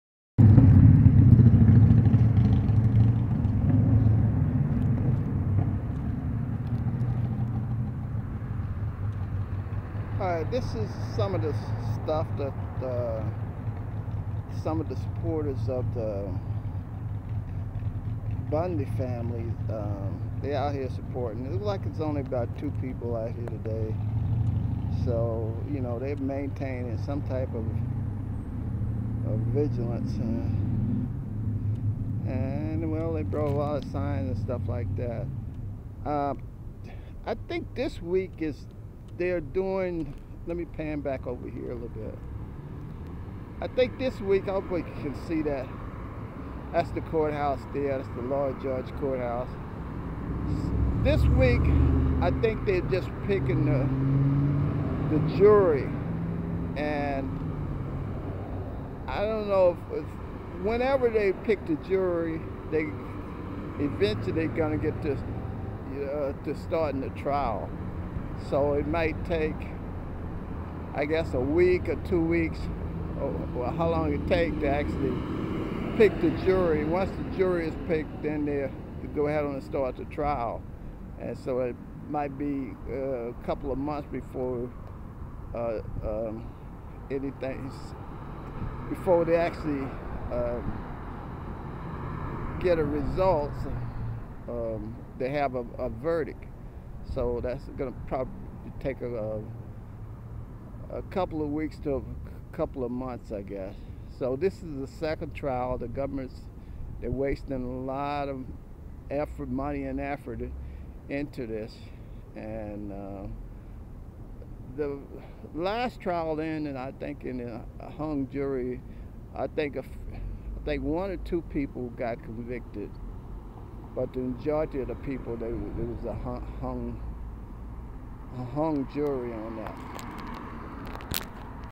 Cliven Bundy retrial:
defendants in the trial of Cliven Bundy supporters will get a retrial after a hung jury a few months ago.